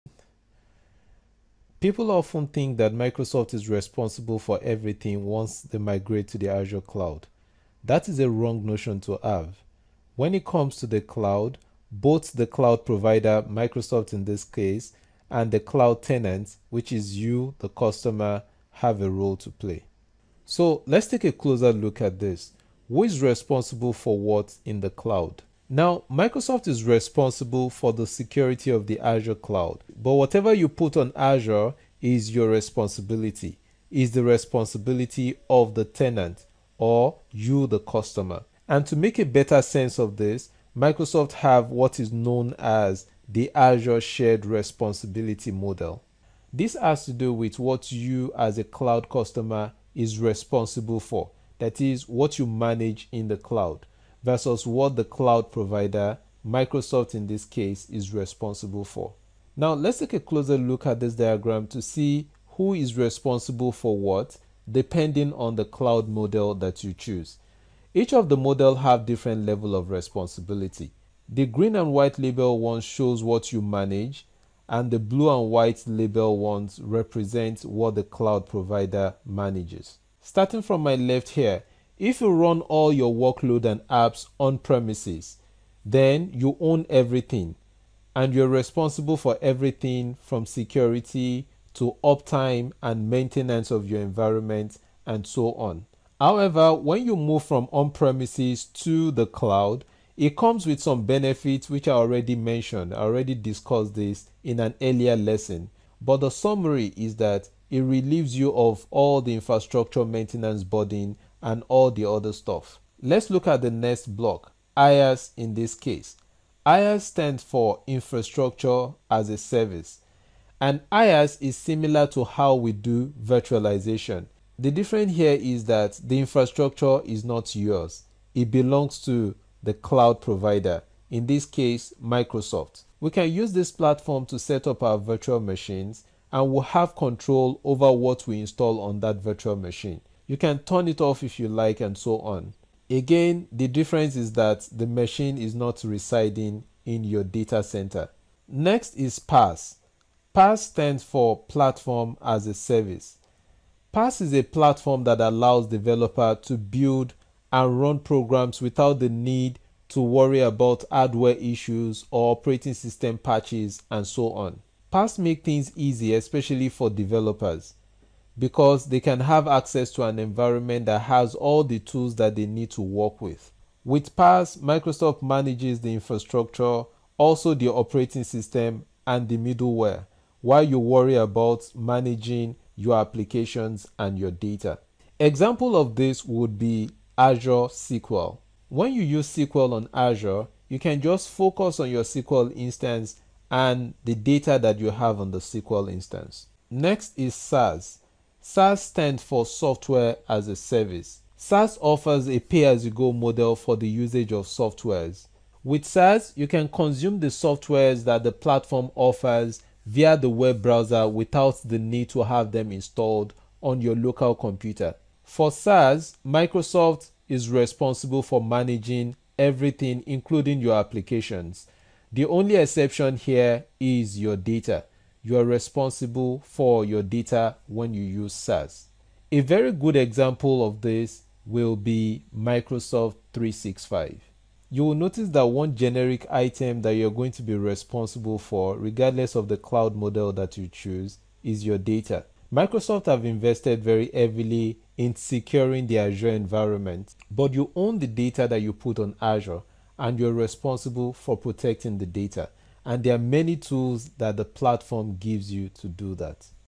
Cloud Computing Services Models - IaaS, PaaS & SaaS
Shared Responsibility Model - Cloud Computing Services Models - IaaS, PaaS & SaaS

The Shared Responsibility Model is a framework that outlines the responsibilities of cloud service providers (CSPs) and customers for every aspect of the cloud environment, including hardware, infrastructure, endpoints, data, etc.